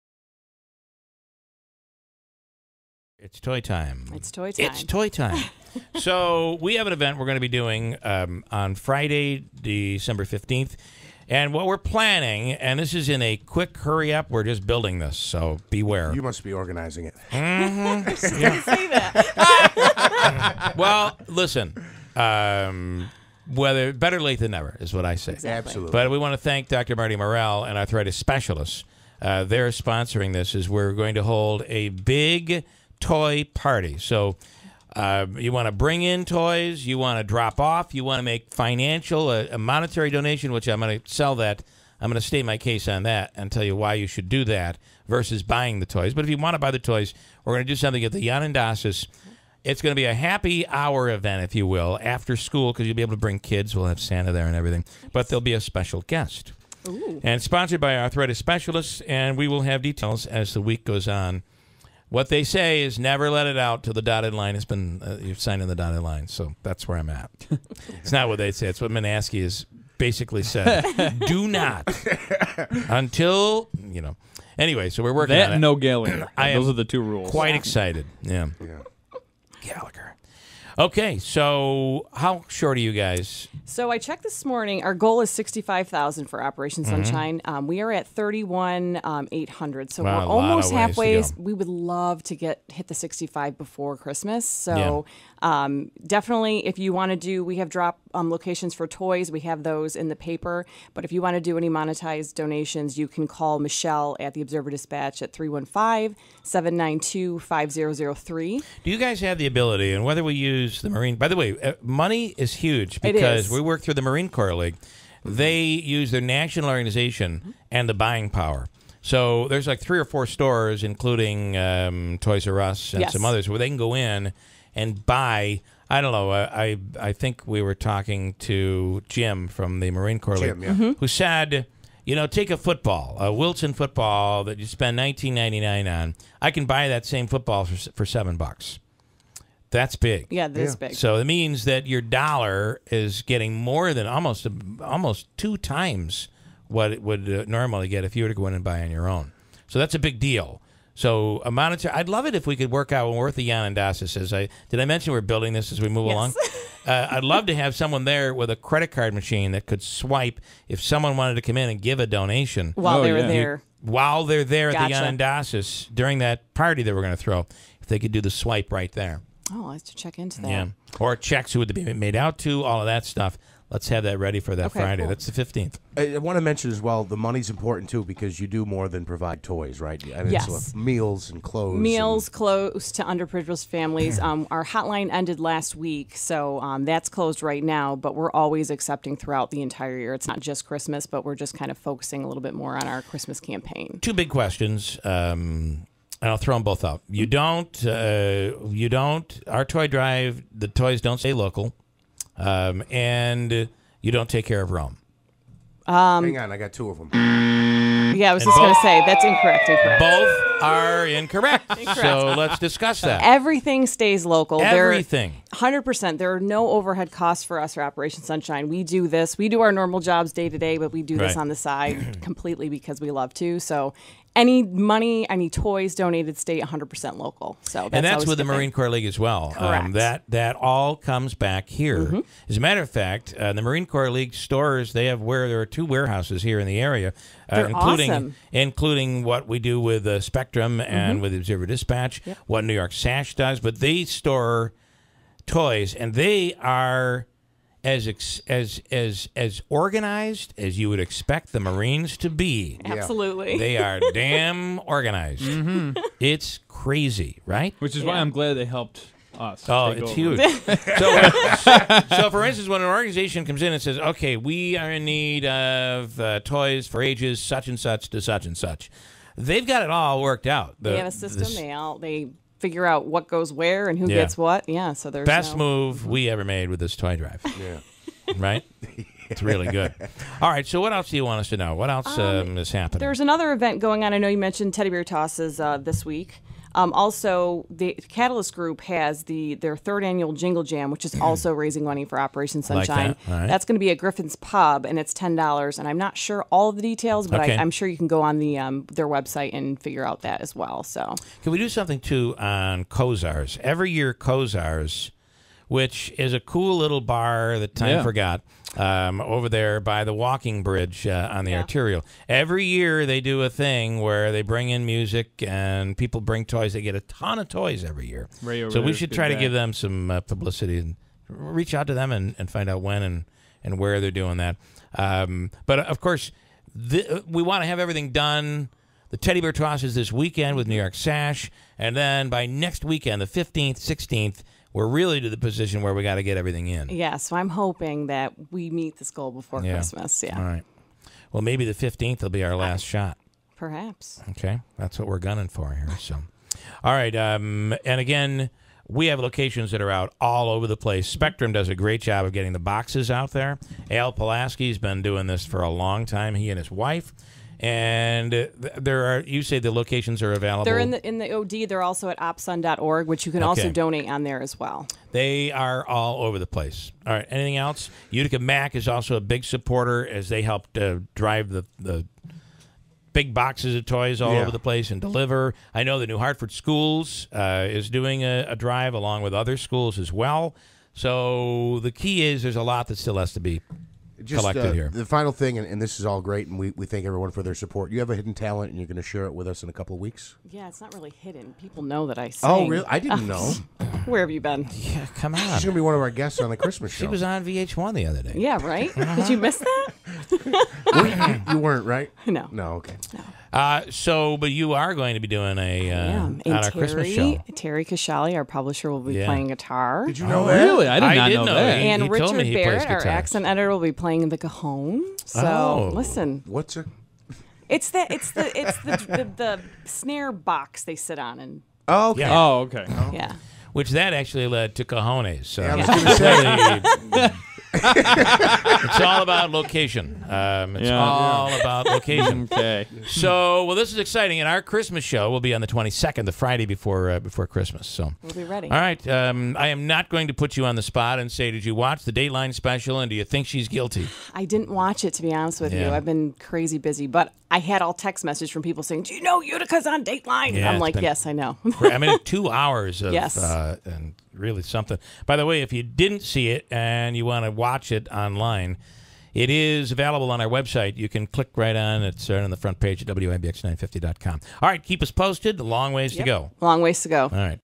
Katie Giacovelli from Utica OD on Operation Sunshine Toy Drive
Katie Giacovelli from the Utica OD is on this morning to talk about the Operation Sunshine Toy Drive and a big event we're holding later this month.